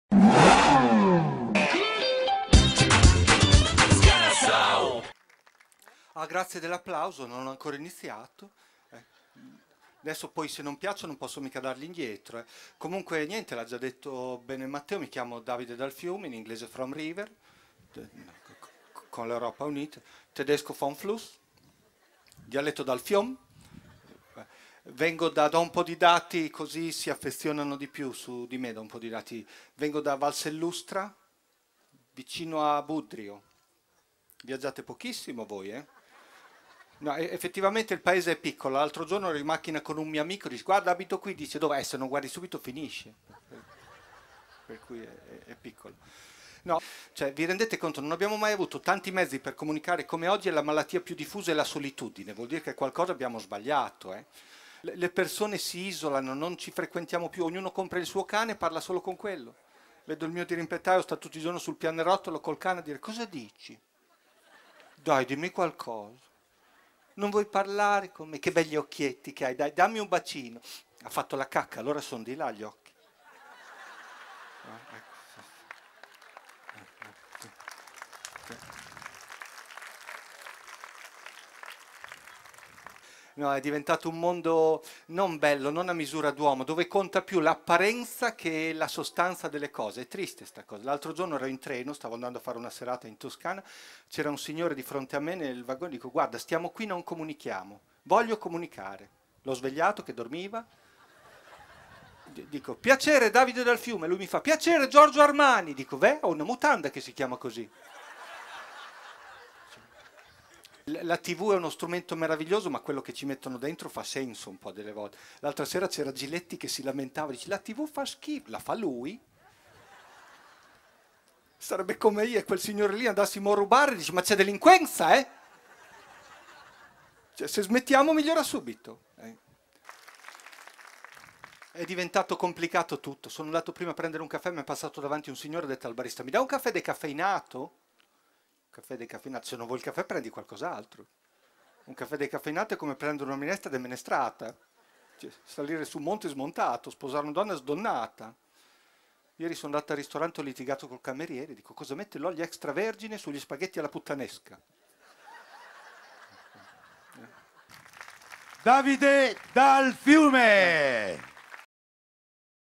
Sganassau 2014 - DAVIDE DAL FIUME
Il comico bolognese premiato allo Sganassau di Bellaria Igea Marina